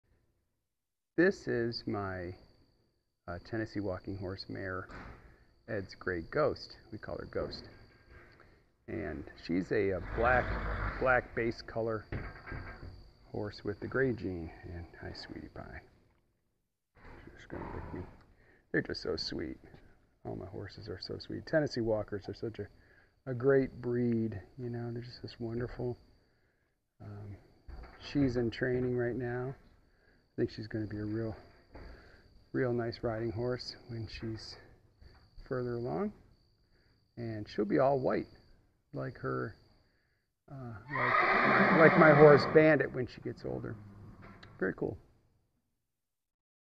My Grey Mare | Her Name is Ghost | Tennessee Walking Horses
This is my Tennessee Walking Horse, Ed's Grey Ghost, we call her Ghost. She is a sweet mare and will make a great riding horse when she is further along in her training.